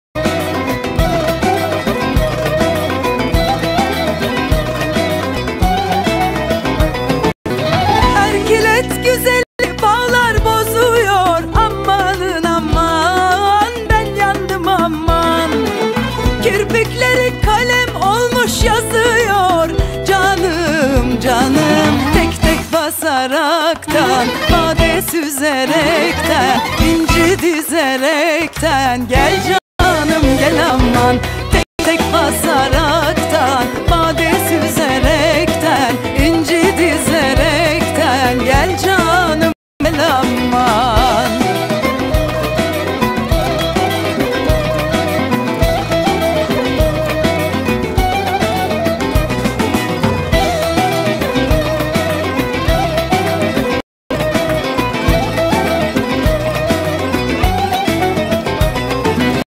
Erkilet Yazır Köyü, Erkilet Bağları
Dede ve Annanenin Bağ evlerine ziyaret